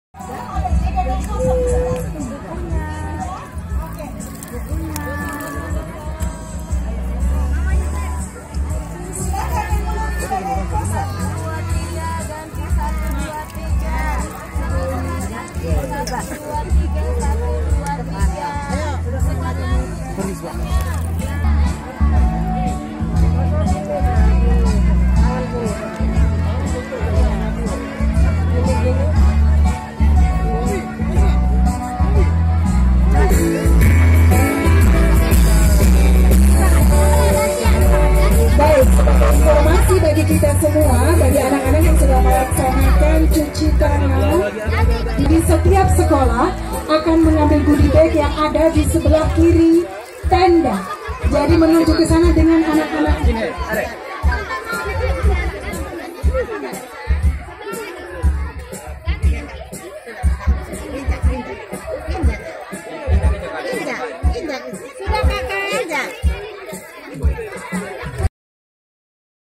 Istri Gubernur NTT Julie Sutrisno Laiskodat Sibuk Ajari Anak Mencuci Tangan. Tonton Videonya
Istri Gubernur NTT Julie Sutrisno Laiskodat  Sibuk Ajari Anak Mencuci Tangan. Hal itu dilakukan pada moment memperingati Hari Cuci Tangan Sedunia Tingkat Provinsi NTT. (POS-KUPANG.COM)